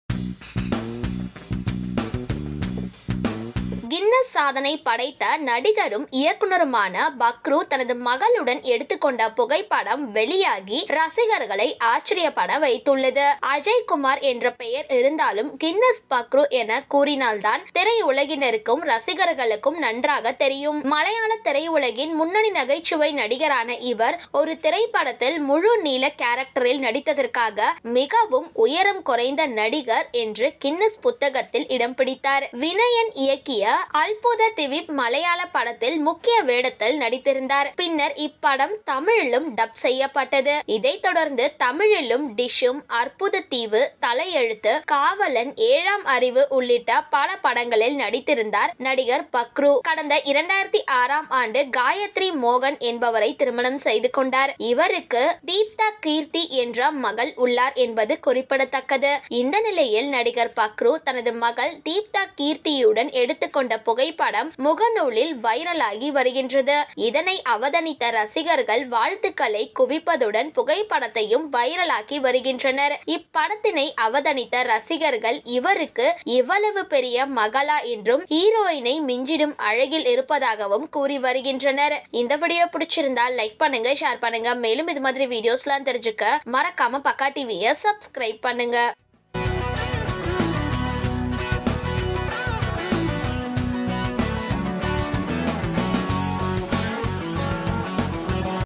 நடிகர் பக்ரூவிற்கு இவ்வளவு பெரிய மகளா? ஹீரோயினை மிஞ்சிய அழகு | Guinness Pakru Daughter
நடிகர் பக்ரூவிற்கு இவ்வளவு பெரிய மகளா? ஹீரோயினை மிஞ்சிய அழகில் வெளியான போட்டோ | Guinness Pakru Daughter